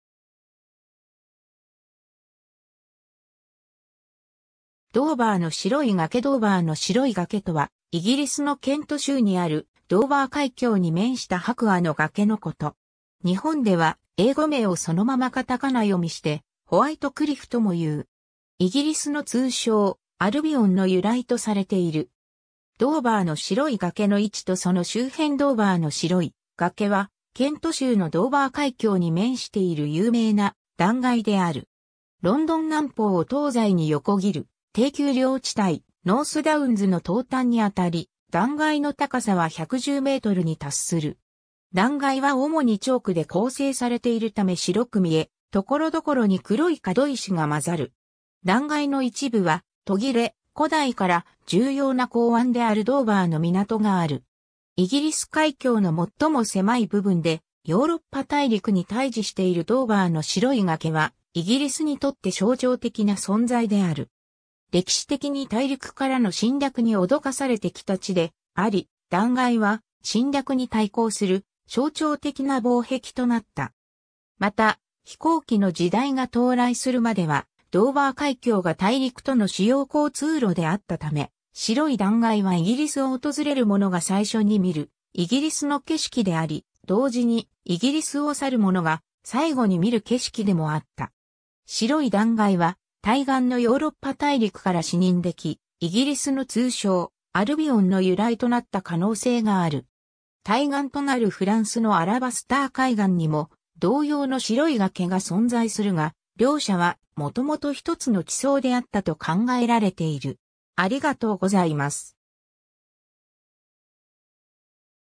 ドーバーの白い崖
ドーバーの白い崖（2012年7月） ドーバーの白い崖 （ドーバーのしろいがけ、White Cliffs of Dover ）とは、イギリスのケント州にあるドーバー海峡に面した白亜の崖のこと。
日本では、英語名をそのまま片仮名読みして「ホワイト・クリフ」ともいう。
イギリスの通称「アルビオン」の由来とされている。
 ドーバーの白い崖の位置とその周辺 ドーバーの白い崖は、ケント州のドーバー海峡に面している有名な断崖である。
ロンドン南方を東西に横切る低丘陵地帯ノース・ダウンズの東端にあたり、断崖の高さは110メートル（350フィート）に達する。
断崖は主にチョーク（白亜）で構成されているため白く見え、所々に黒い燧石が混ざる。
断崖の一部は途切れ、古代から重要な港湾であるドーバーの港がある。
 イギリス海峡の最も狭い部分でヨーロッパ大陸に対峙しているドーバーの白い崖は、イギリスにとって象徴的な存在である。
歴史的に大陸からの侵略に脅かされてきた地であり、断崖は侵略に対抗する象徴的な防壁となった。
また、飛行機の時代が到来するまではドーバー海峡が大陸との主要交通路であったため、白い断崖はイギリスを訪れる者が最初に見るイギリスの景色であり、同時にイギリスを去る者が最後に見る景色でもあった。
白い断崖は対岸のヨーロッパ大陸から視認でき、イギリスの通称「アルビオン」（ラテン語で「白い」の意味の「albus」が語源。）の由来となった可能性がある。
対岸となるフランスのアラバスター海岸（英語: Côte d'Albâtre）にも同様の白い崖が存在するが、両者はもともと一つの地層であったと考えられている。